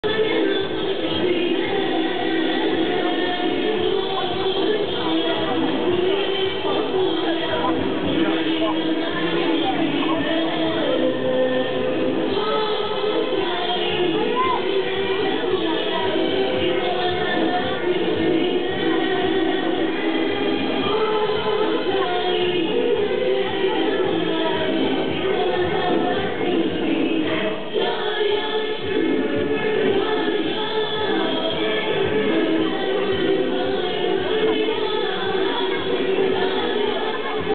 Uighur dance
中國西南民族大學舞蹈團 Southwest University of National Minorities of China performs a 維吾爾/Uighur dance in gold lame high heels. Wulai, Taiwan, November 7, 2006